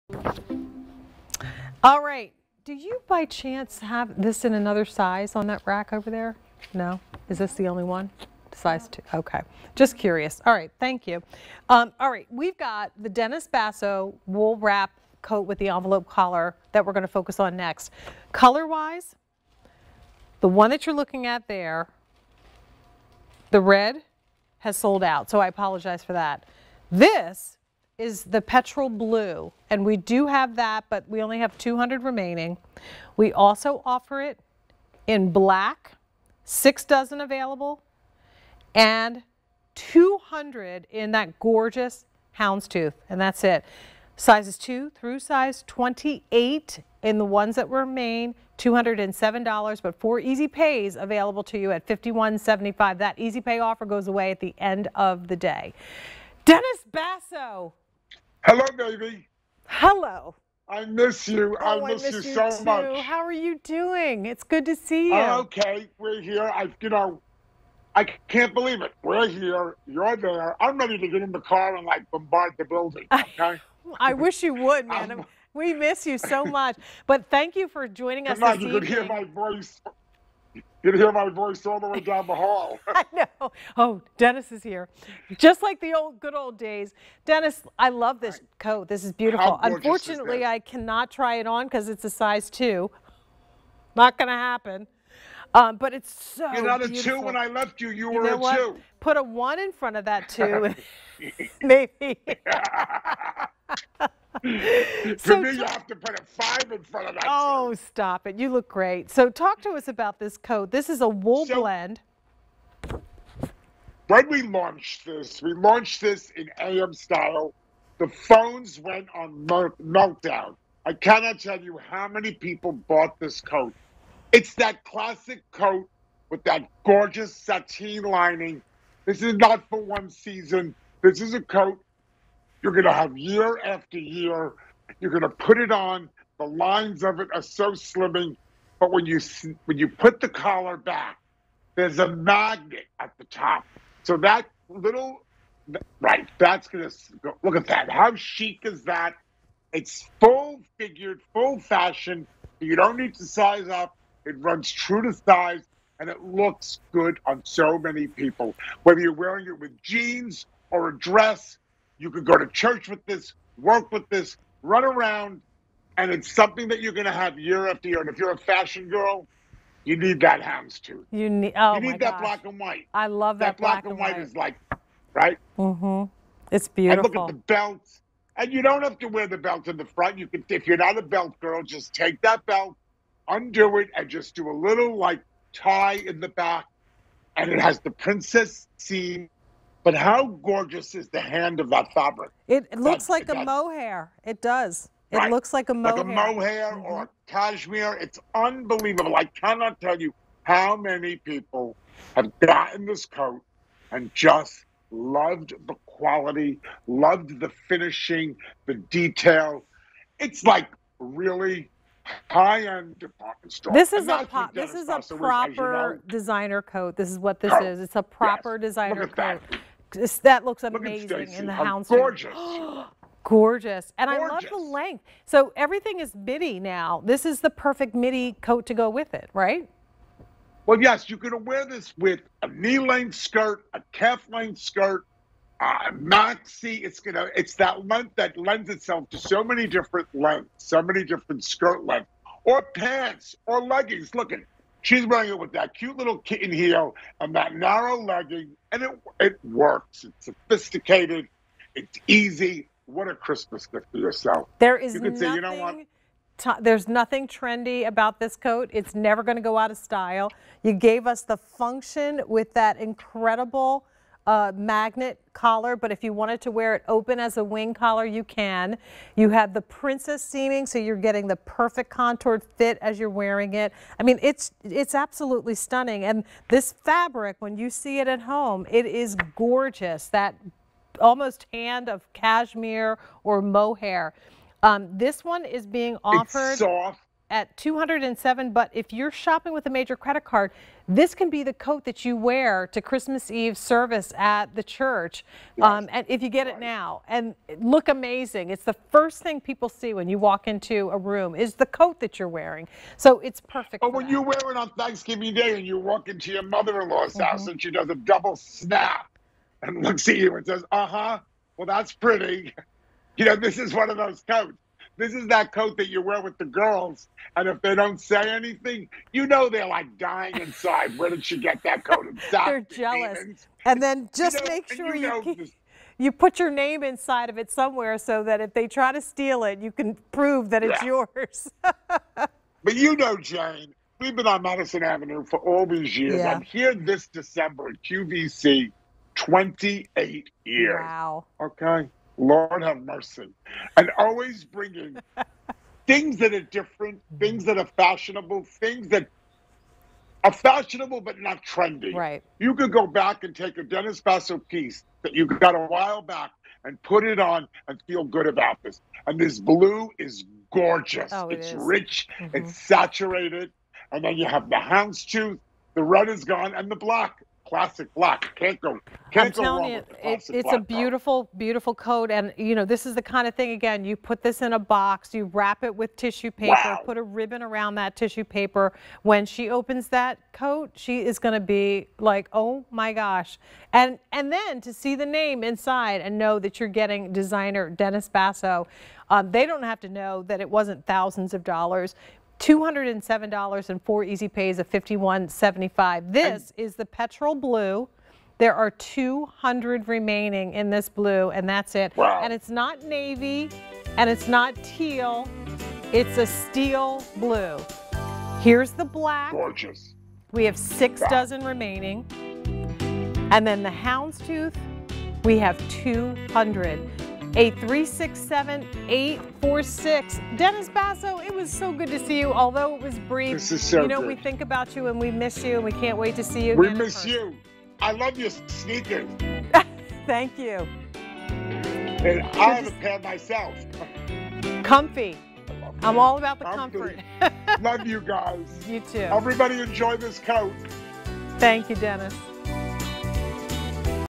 Dennis Basso Wool Blend Wrap Coat with Envelope Collar on QVC
Dennis Basso Wool Blend Wrap Coat with Envelope Collar

Keep warm in this wool wrap jacket. An envelope collar and belted waist are feminine features that give it a pretty, polished look. From Dennis Basso.